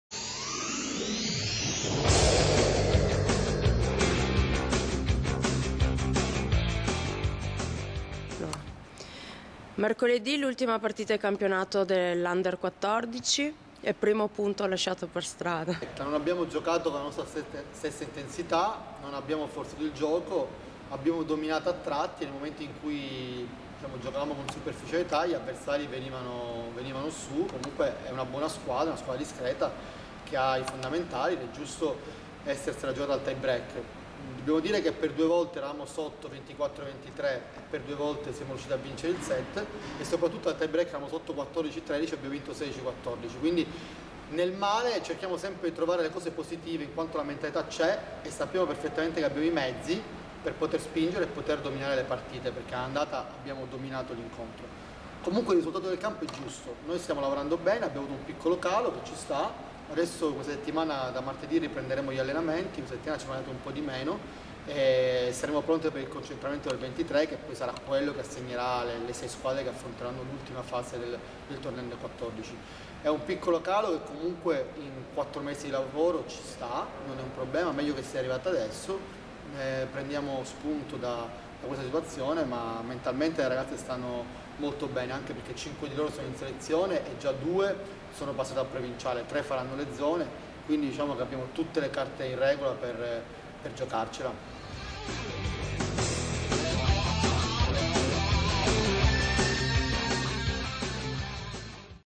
Chiusa la prima parte del campionato under14
Il punto di coach Tagliabue.
Domenica 23 il concentramento.